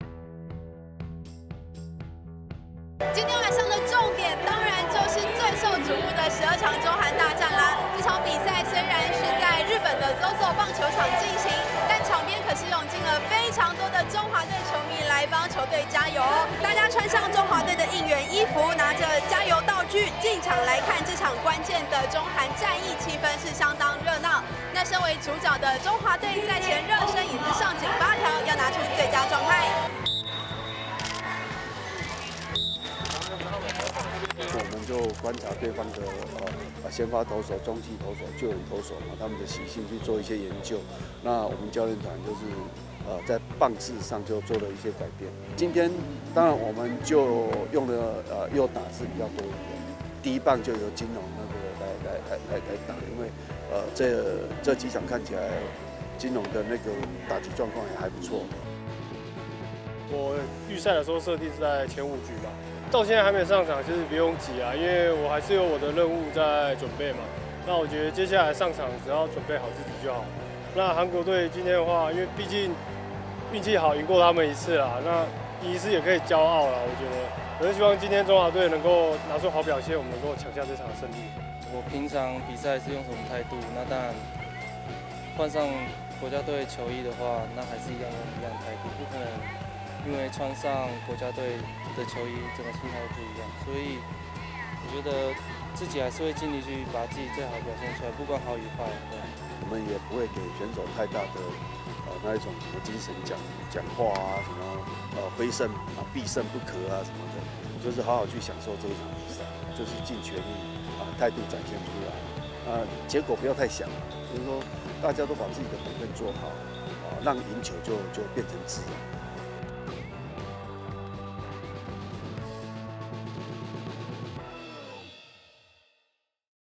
12強 / 中華隊調整打線出戰韓國 台灣球迷進場應援到底
眾所矚目的台韓大戰來啦！
洪總在賽前也將先發打序再次作出調整！
目前看來很有成效！